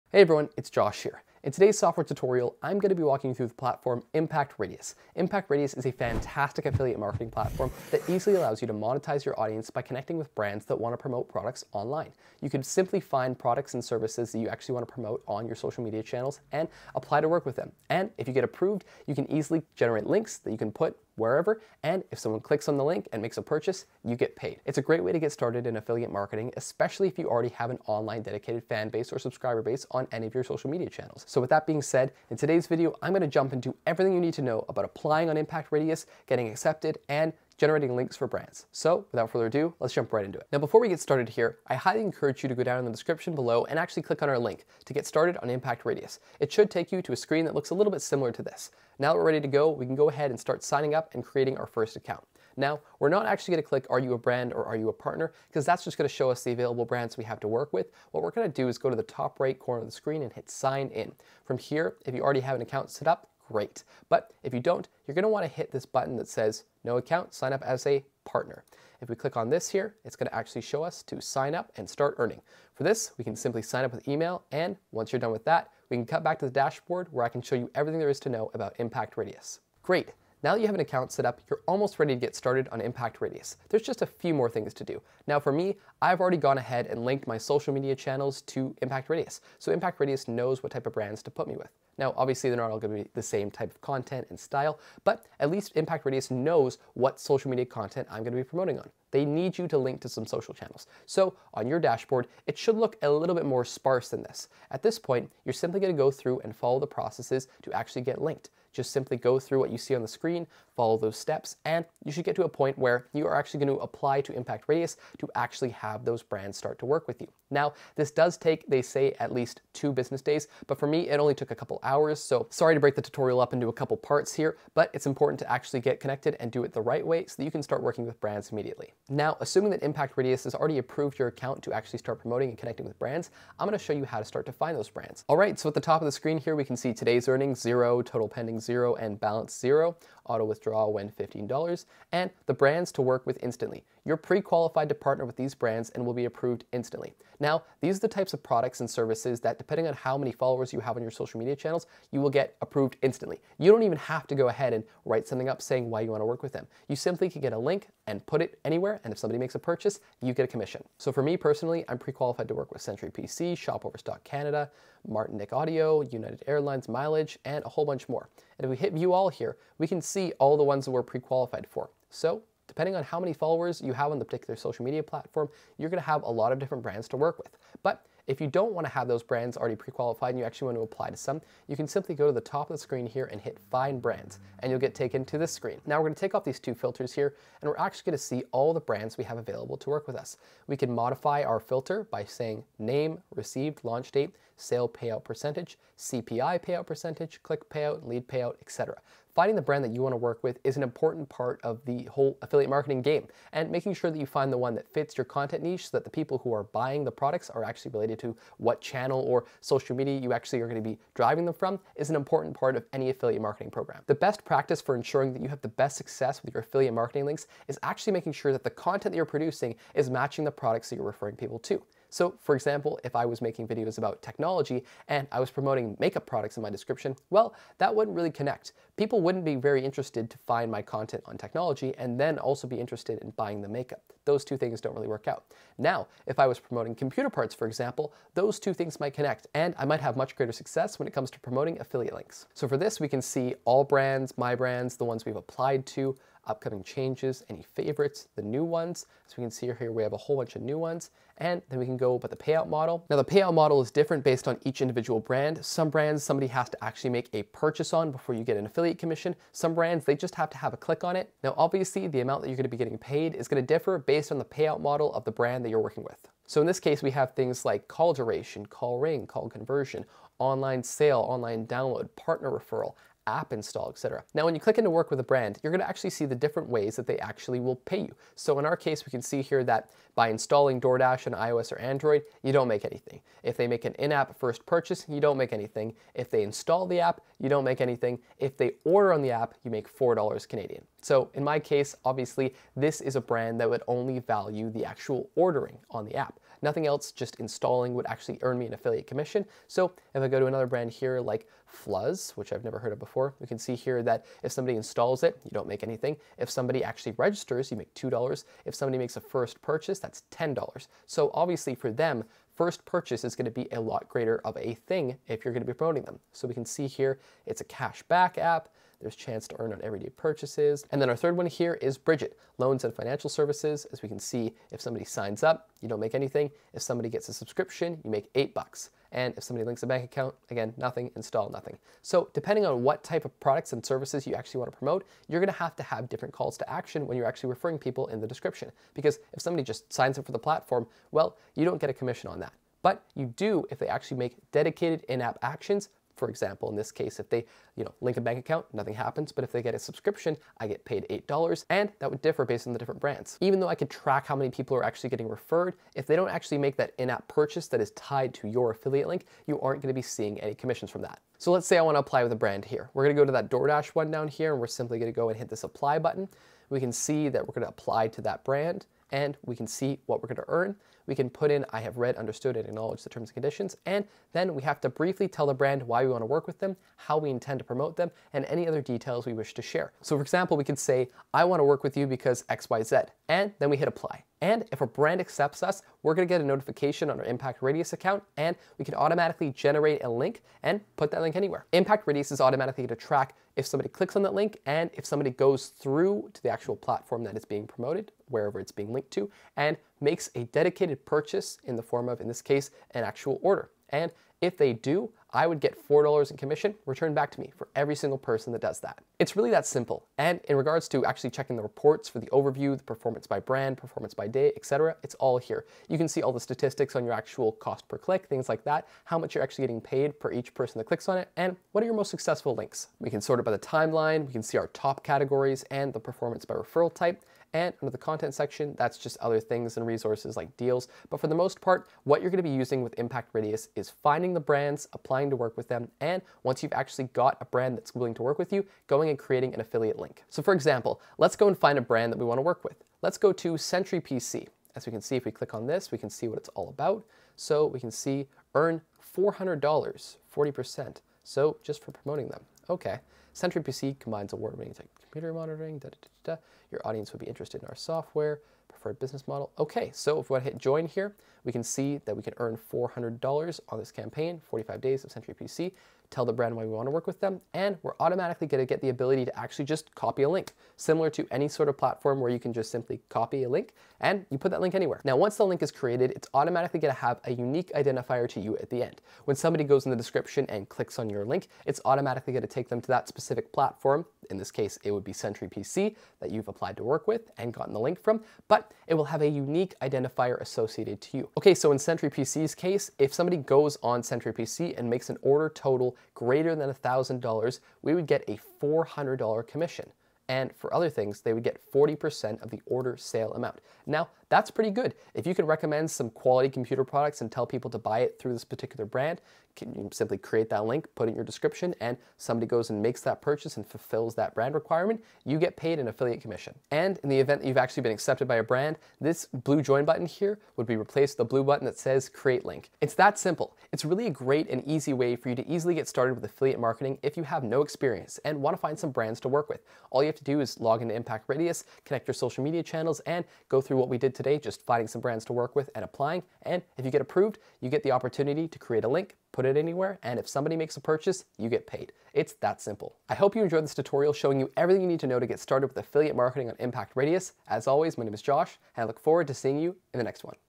How To Use Impact Radius For Affiliate Marketing 2024 (Step by Step Guide)
In this video, Josh shows you how to use Impact for affiliate marketing in 2024. Impact is the leading platform for affiliate marketing.